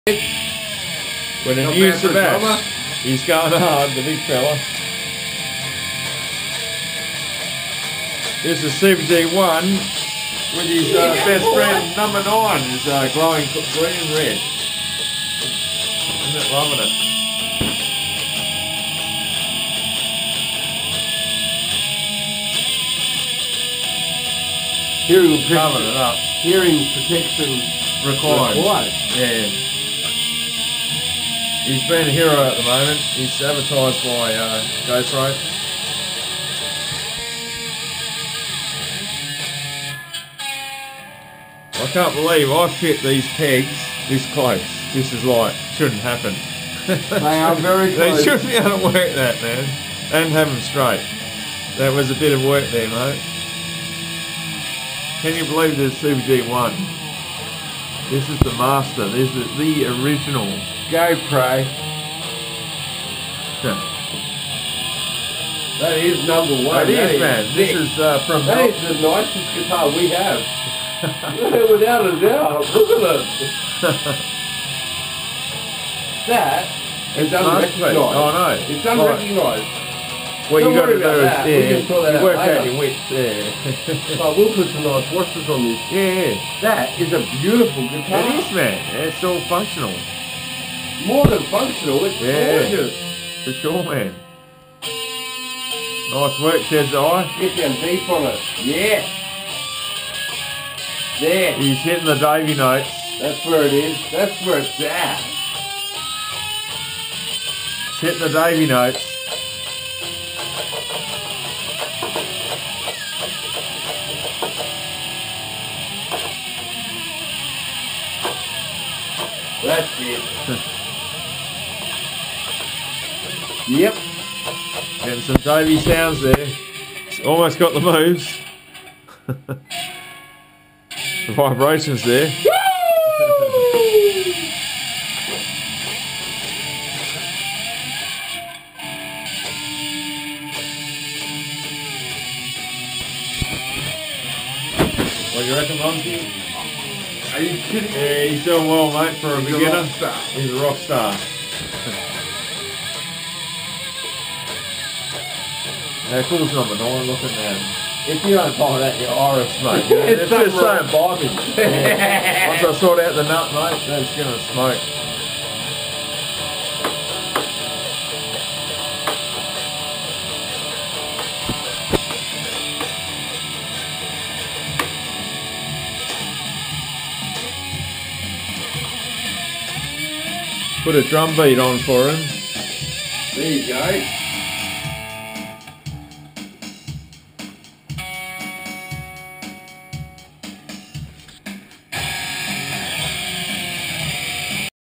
Shedz on CBG1 take 1
Shedz is carving up CBG1 with a few Dave Hole licks there, good work big guy-i !!